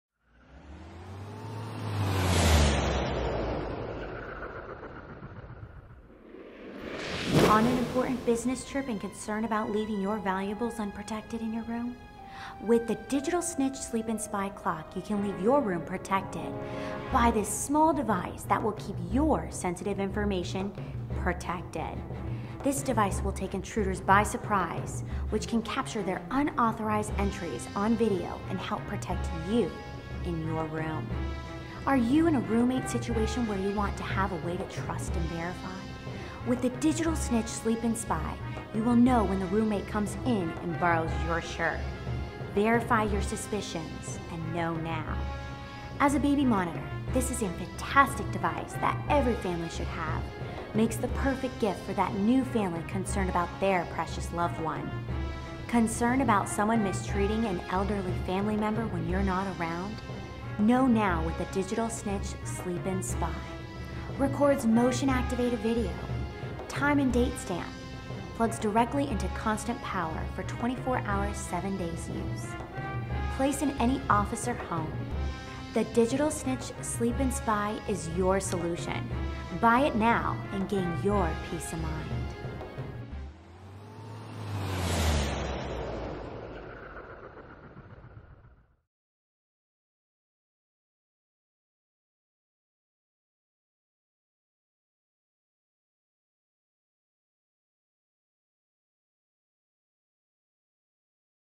Digital Snitch Sleep & Spy Clock
- Records Video
- Motion Recording
- Time and date stamp
- Plug directly into constant power for 24 hours 7 days use

Have a witness, use the digital snitch sleep and spy clock.

On an important business trip and concerned about leaving your valuables unprotected in the room? With the digital snitch sleep and spy clock you can leave your room protected by a small device that will keep your sensitive equipment or information protected.  This device will take intruders by surprise which can capture there unauthorized entries on video to help you protect your valuables and sensitive information.

Are you in the roommate situation where you want to have a way to trust and verify with the digital snitch sleep and spy you will know when the roommate comes in to borrow your shirt.  Verify your suspicions and know now.

As a baby monitor this is a fantastic device that every family should have.
Makes the perfect gift for that new family concerned about there precious loved one.

Concerned about someone mistreating an elderly family member when your not around?

Concerned about your pets doing things that they shouldn't be doing?

The simple, compact, travel device

-The Digital Snitch Sleep & Spy is your SOLUTION
- Record motion activated video